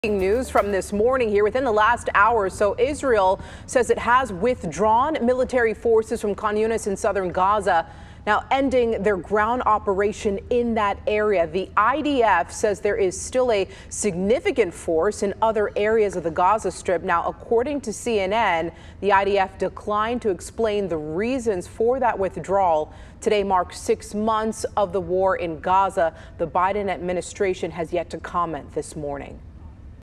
Report: Israeli military reduces troops in southern Gaza, spokesperson says
The Israeli military has withdrawn more ground troops from the southern Gaza Strip.